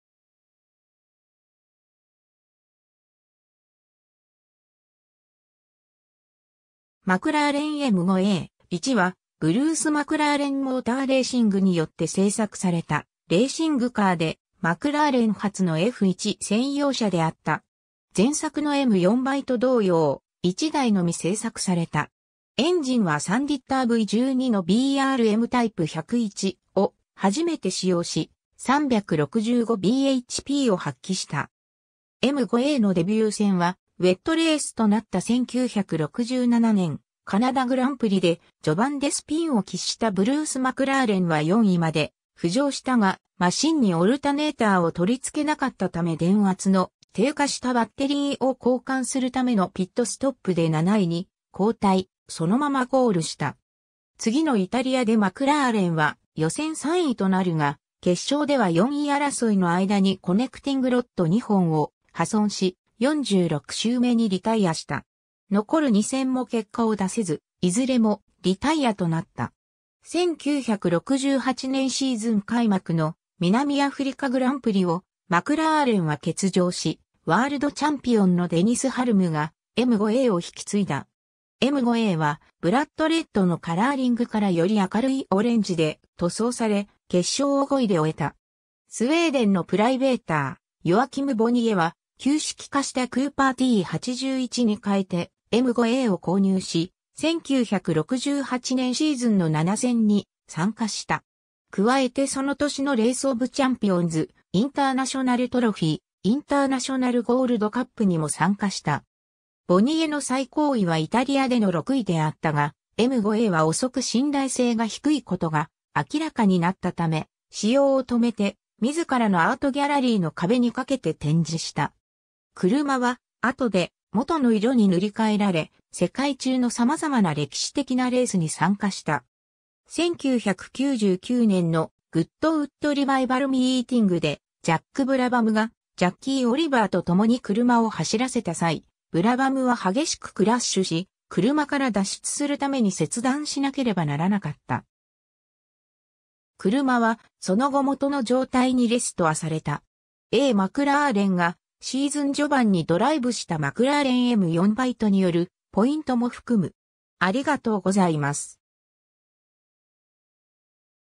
マクラーレン・M5A
マクラーレン・M5A/1 (McLaren M5A/1) は、ブルース・マクラーレン・モーターレーシングによって製作されたレーシングカーで、マクラーレン初のF1専用車であった。
前作のM4B同様、1台のみ製作された。
エンジンは3リッターV12のBRM・タイプ101を初めて使用し、365 bhpを発揮した。
 M5Aのデビュー戦はウェットレースとなった1967年カナダグランプリで、序盤でスピンを喫したブルース・マクラーレンは4位まで浮上したが、マシンにオルタネーターを取り付けなかったため電圧の低下したバッテリーを交換するためのピットストップで7位に後退、そのままゴールした。
次のイタリアでマクラーレンは予選3位となるが、決勝では4位争いの間にコネクティングロッド2本を破損し、46周目にリタイアした。
残る2戦も結果を出せず、いずれもリタイアとなった。
 1968年シーズン開幕の南アフリカグランプリをマクラーレンは欠場し、ワールドチャンピオンのデニス・ハルムがM5Aを引き継いだ。
M5Aはブラッド・レッドのカラーリングからより明るいオレンジで塗装され、決勝を5位で終えた。
 スウェーデンのプライベーター、ヨアキム・ボニエは旧式化したクーパー・T81に換えてM5Aを購入し、1968年シーズンの7戦に参加した。
加えてその年のレース・オブ・チャンピオンズ、インターナショナルトロフィー、インターナショナルゴールドカップにも参加した。
ボニエの最高位はイタリアでの6位であったが、M5Aは遅く信頼性が低いことが明らかになったため、使用を止めて自らのアートギャラリーの壁に掛けて展示した。
 車は後で元の色に塗り替えられ、世界中の様々な歴史的なレースに参加した。
1999年のグッドウッド・リバイバル・ミーティングでジャック・ブラバムがジャッキー・オリバーと共に車を走らせた際、ブラバムは激しくクラッシュし、車から脱出するために切断しなければならなかった。
車はその後元の状態にレストアされた。
 (key) （太字はポールポジション、斜体はファステストラップ） [a] ^ マクラーレンがシーズン序盤にドライブしたマクラーレン・M4Bによるポイントも含む。